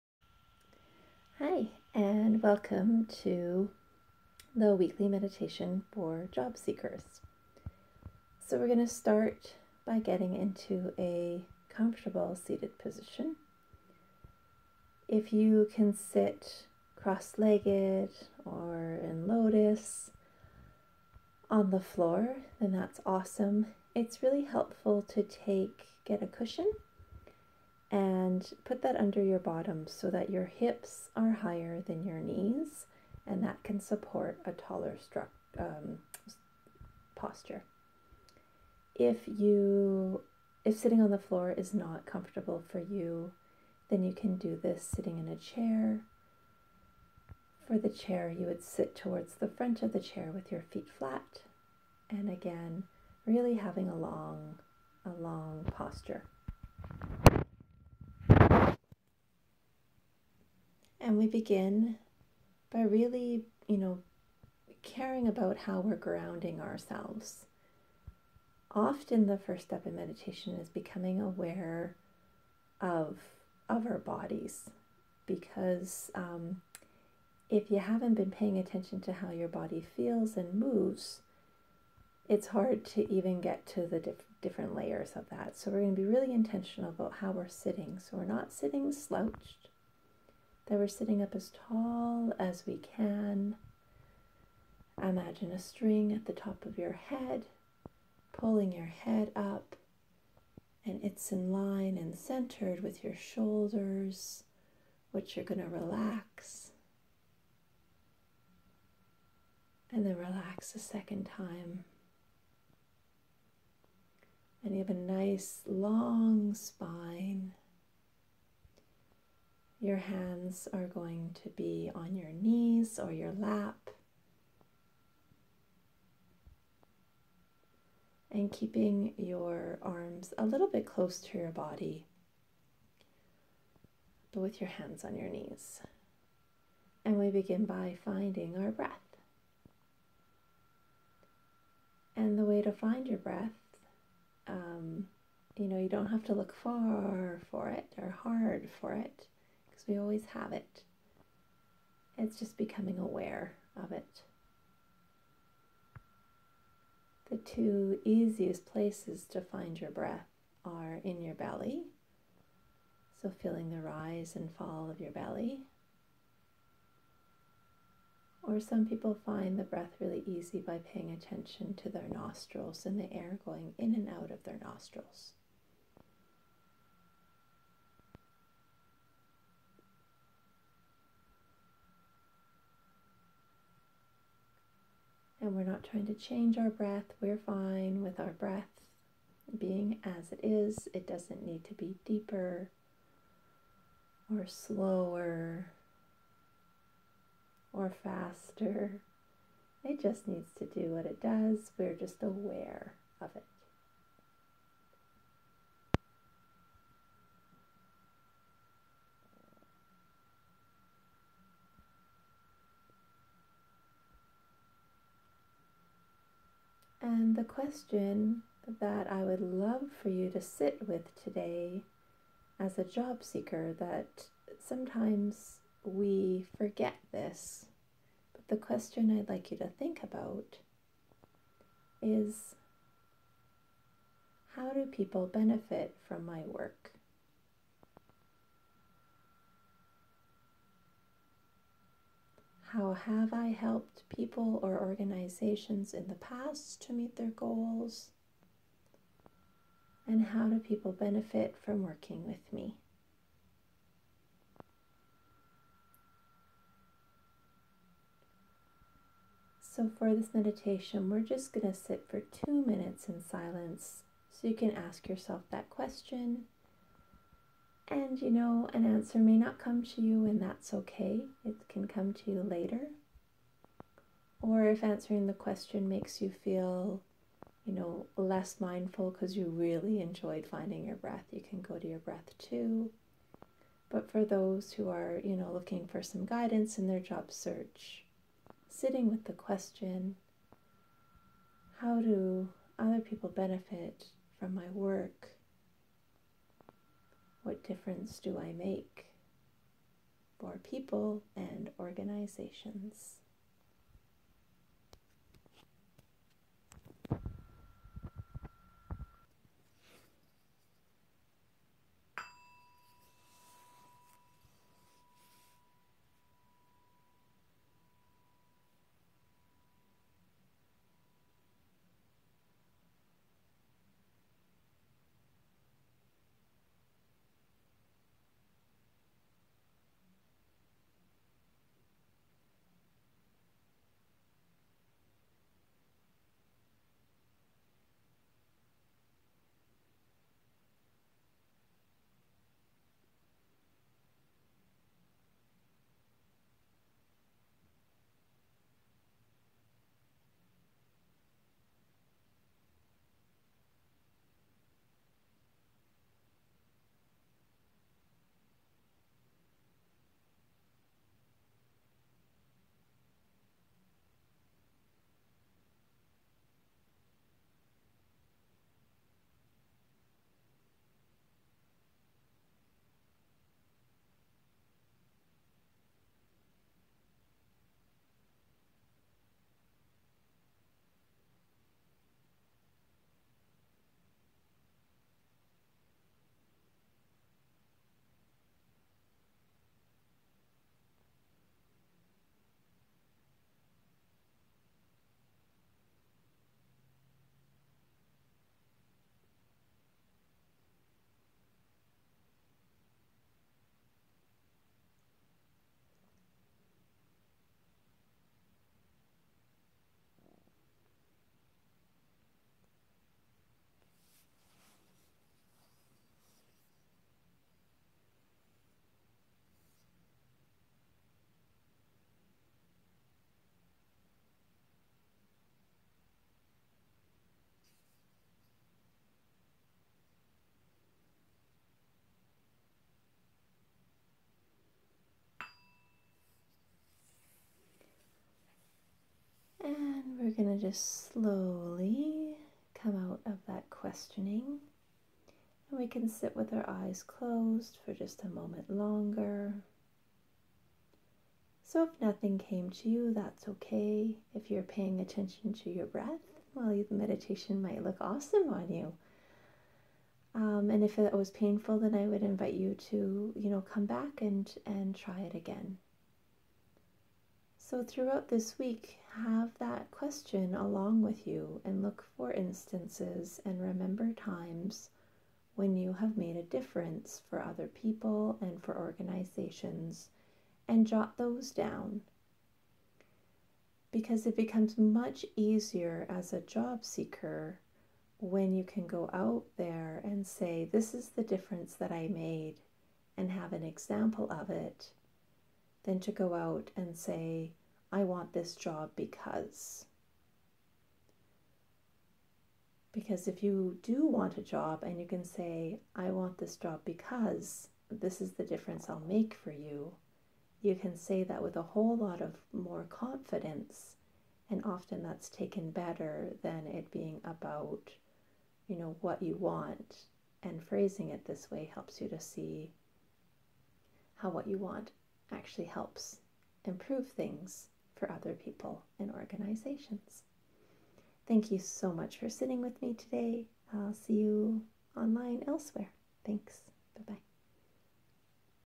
Meditation for Job Seekers: What value do you bring?
Weekly meditation for job seekers to help focus your search.

In this meditation, we sit, watch our breath & identify how other people and organizations benefit from our work.